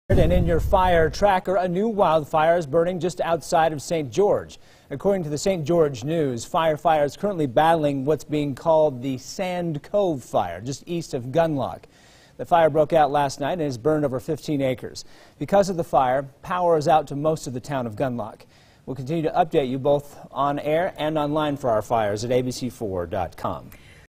NEW FIRE: Sand Cove Fire
Fire broke out Sunday night, causing power outages in Gunlock, Utah.